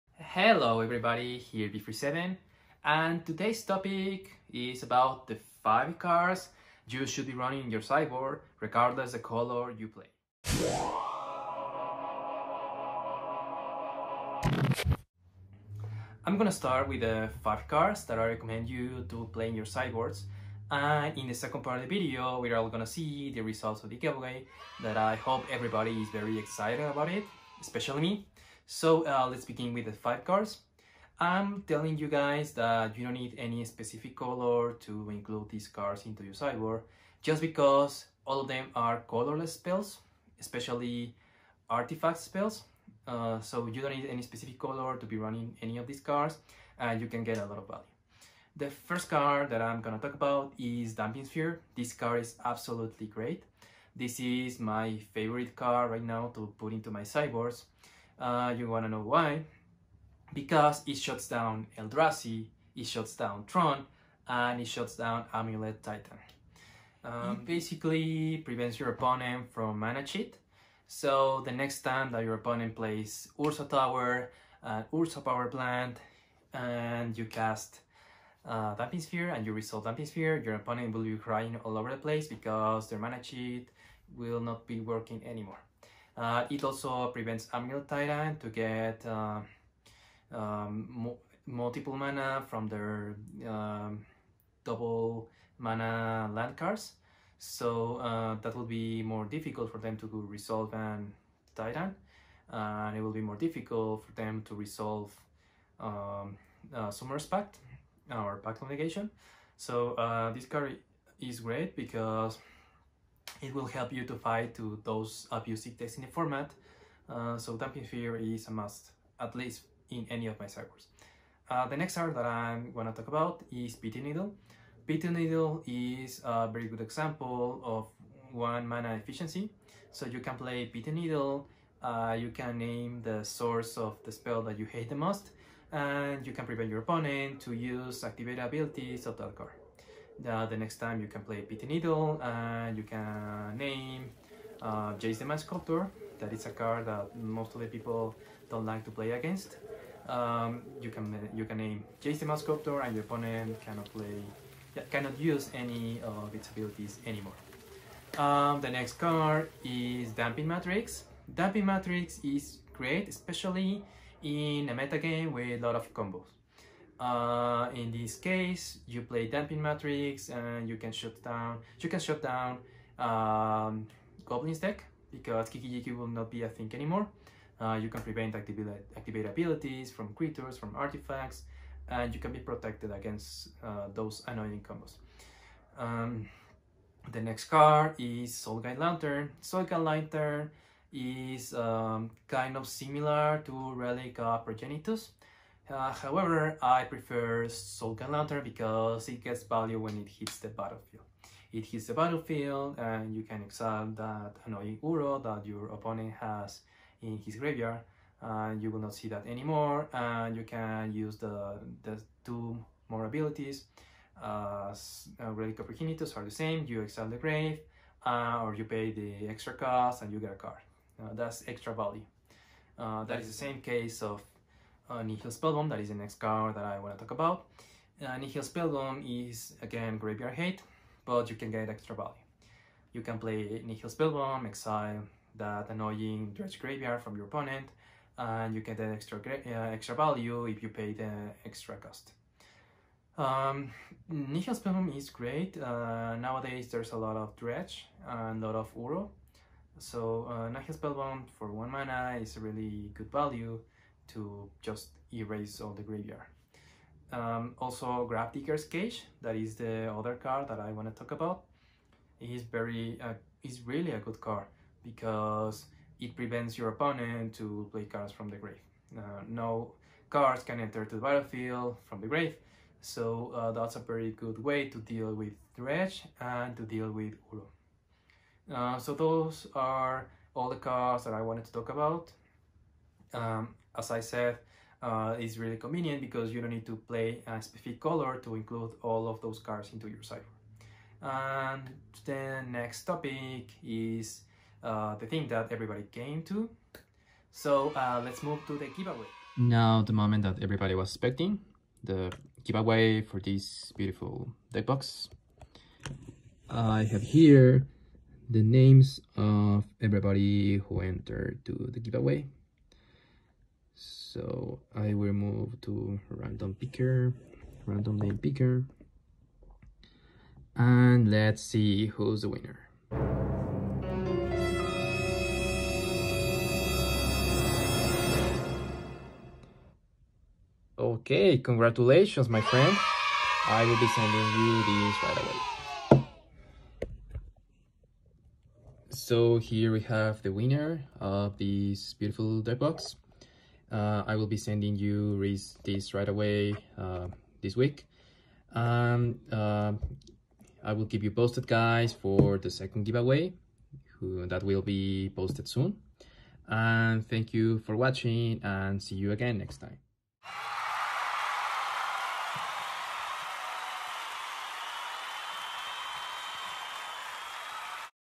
Five cards you should have in your sideboard (Modern) + giveaway results!
Welcome to another topic video! This time the 5 cards I consider staples in modern atm, you can play any of this regardless the color you play!

For the giveaway winner I will be contacting you via Instagram account to make everything smooth! Congratulations 🥳🥳 Thanks to everybody who participated and see you again soon for the next giveaway!